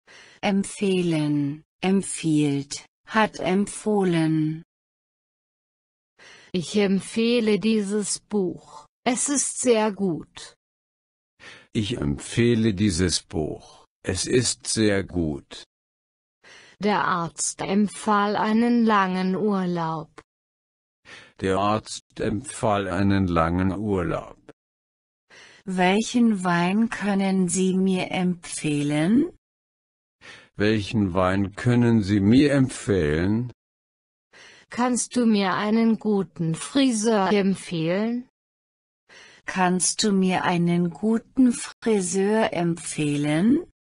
Wortschatz A2: empfehlen
คำศัพท์ A2: empfehlen, empfiehlt, hat empfohlen
Ich empfehle dieses Buch, es ist sehr gut.
Der Arzt empfahl einen langen Urlaub.
Welchen Wein können Sie mir empfehlen?
Kannst du mir einen guten Friseur empfehlen?